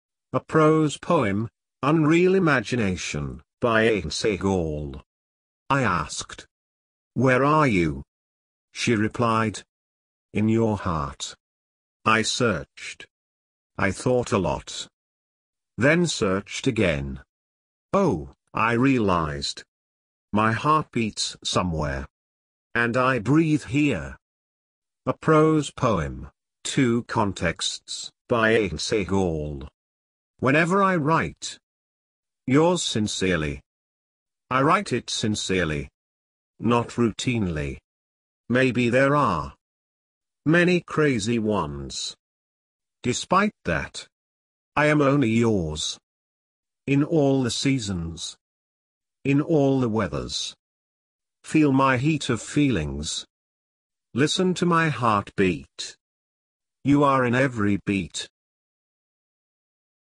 English Poetry | Prose Poems - Unreal Imagination | Two Contexts | Author - Ehsan Sehgal
Two Prose Poems -- By famous poet, author, journalist, and scholar Ehsan Sehgal -- January 2022 -- The Hague, Netherlands.

Unreal Imagination
I asked
Where are you
She replied;

I searched
I thought a lot
Then searched again
Oh, I realized,
My heart beats somewhere
And I breathe here.
- Ehsan Sehgal

Two Contexts
Whenever I write:
"Yours sincerely"
I write it sincerely,
Not routinely

Maybe there are
Many crazy ones
Despite that
I am only yours
In all the seasons
In all the weathers

Feel my heat of feelings
Listen to my heartbeat
You are in every beat.
- Ehsan Sehgal